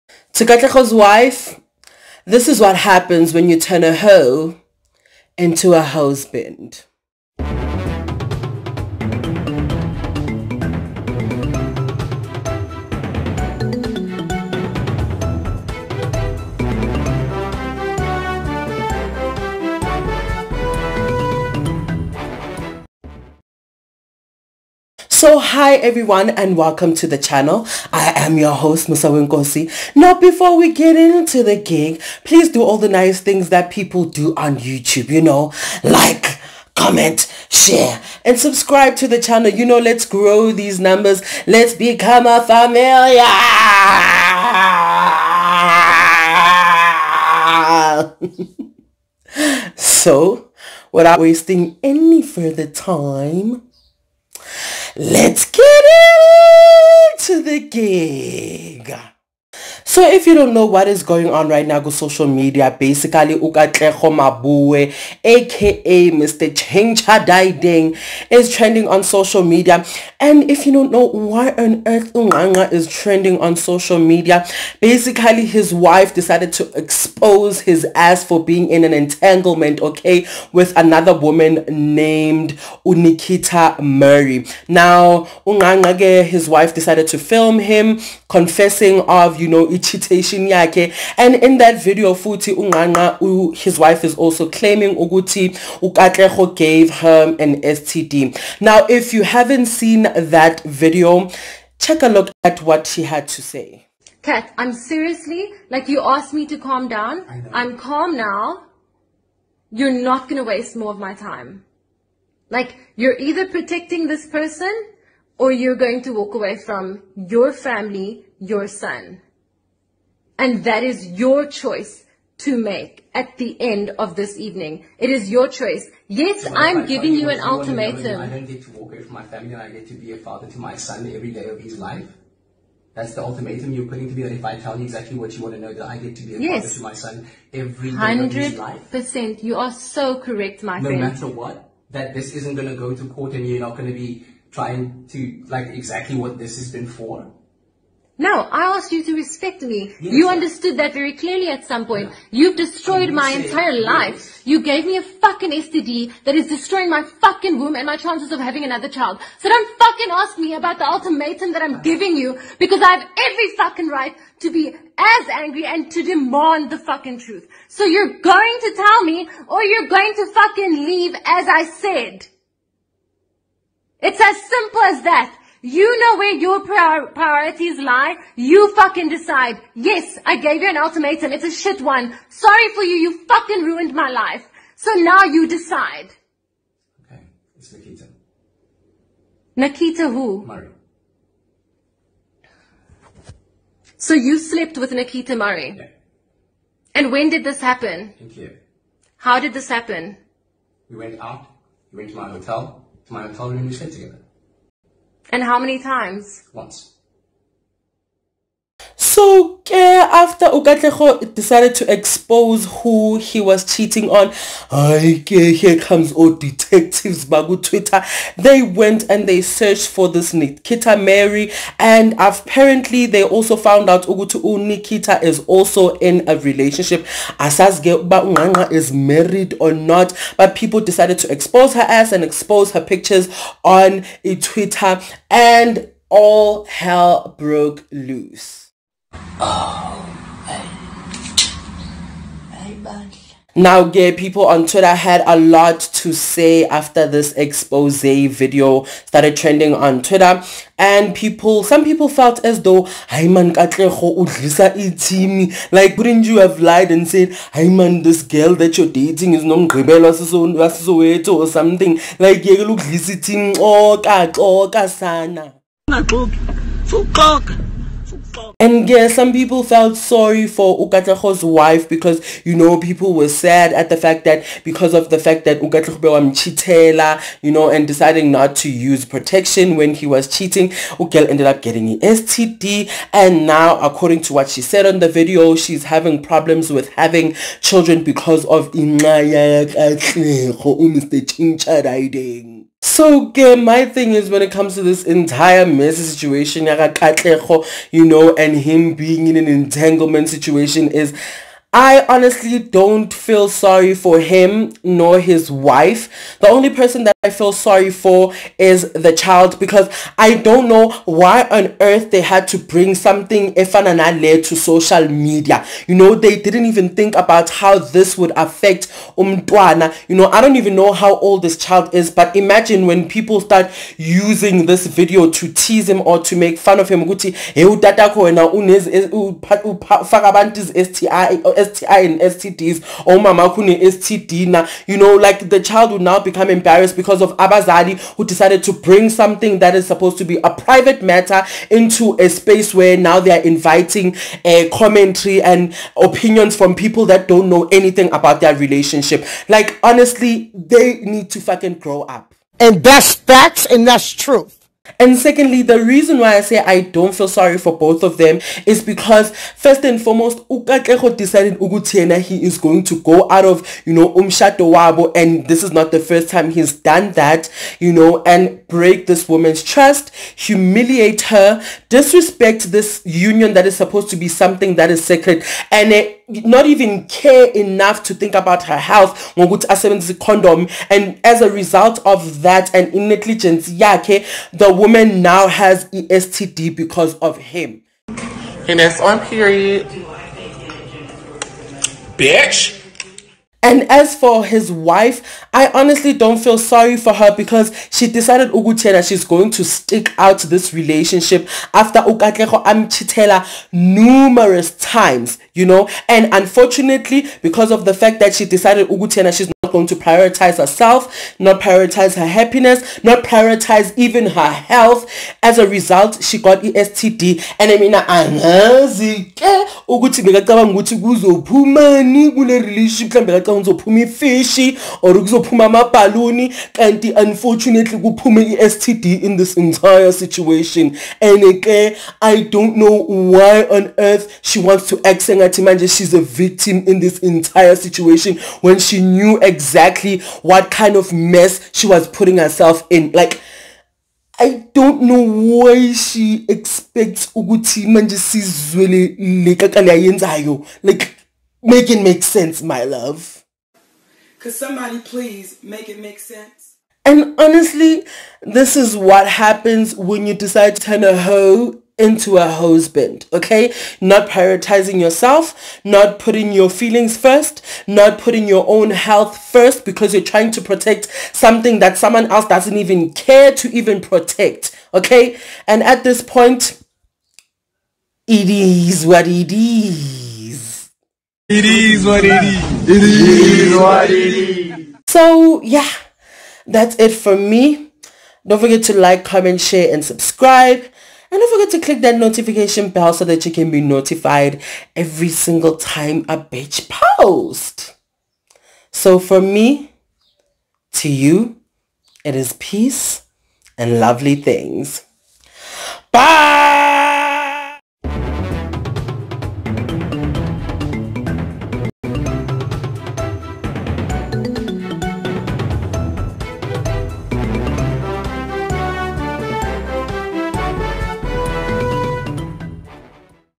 OutSurance actor Katlego Maboe gives wife STD and EXPOSED for cheating. Full Video!!
in this video I'll be talking about KatlegoMaboe and his wife exposing him.